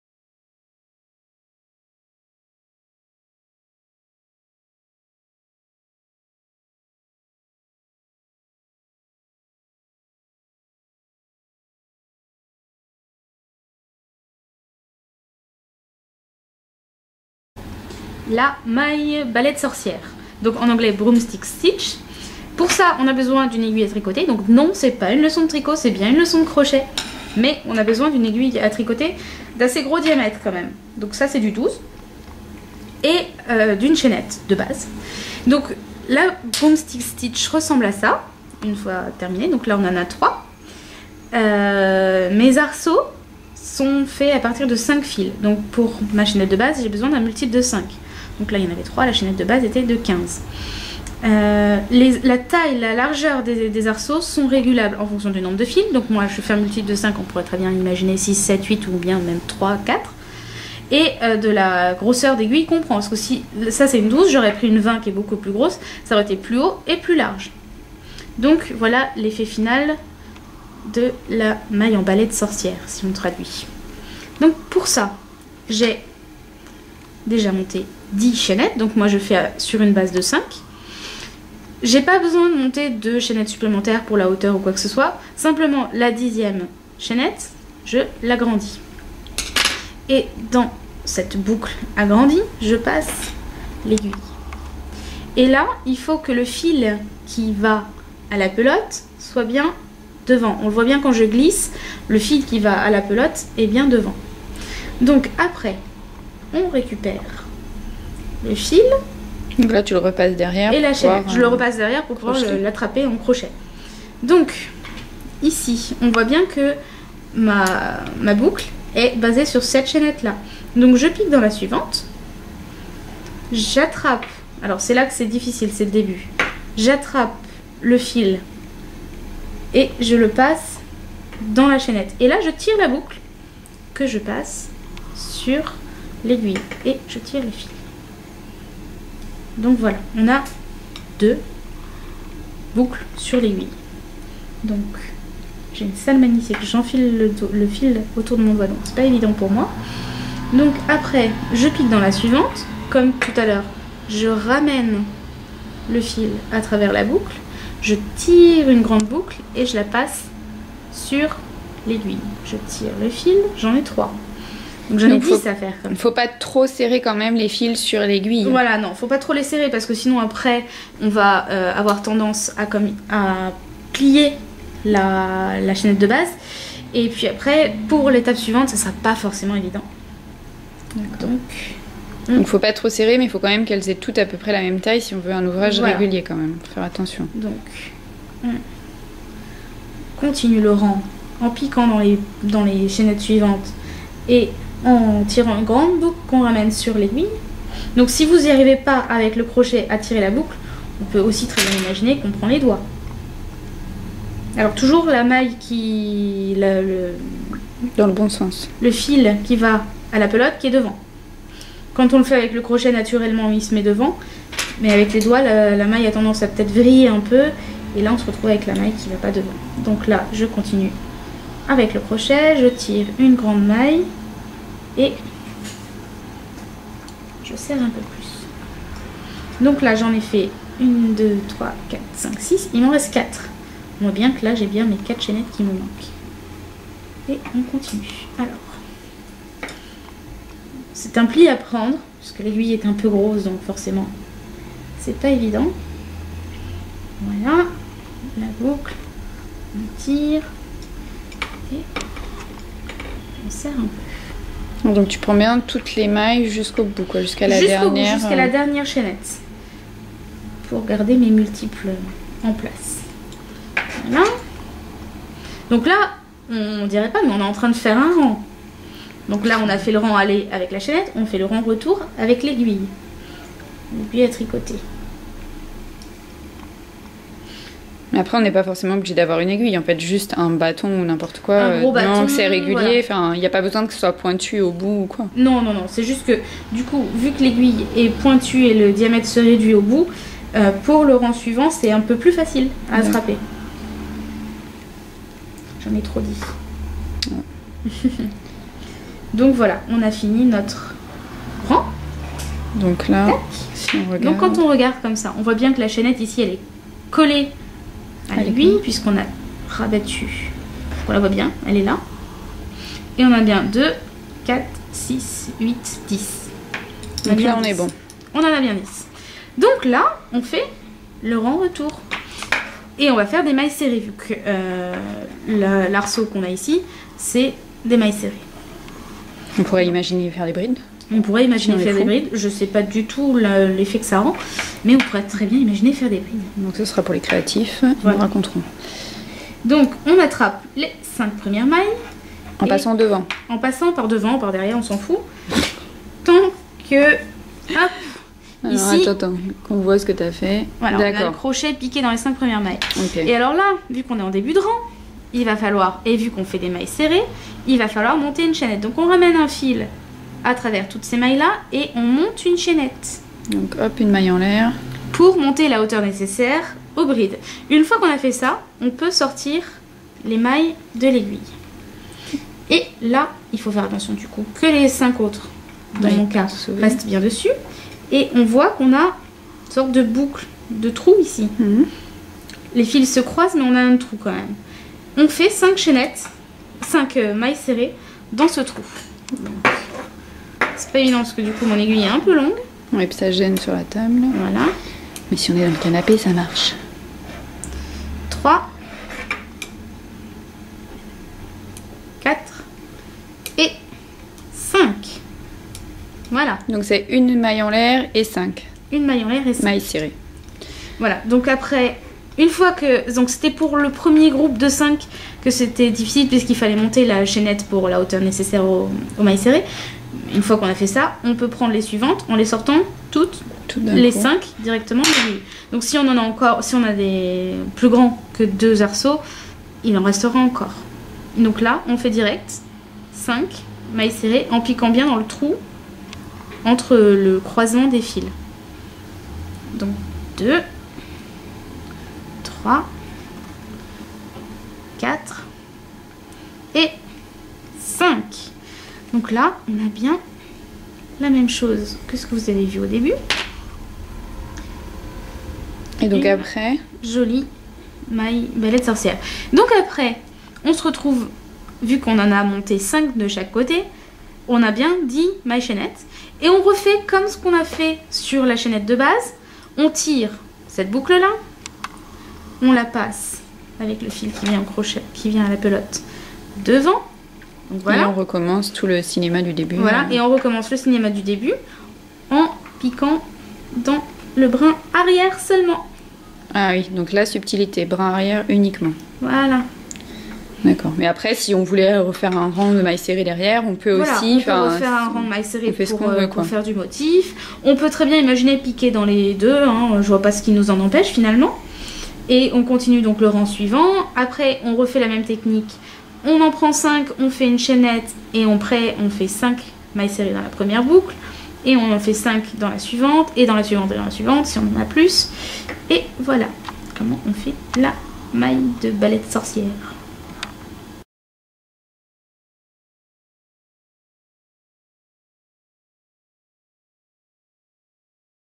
{Crochet} Broomstick stitch
Cette vidéo va vous apprendre à crocheter le point "broomstick stitch".

► Un article correspondant à cette vidéo est disponible avec plus de détails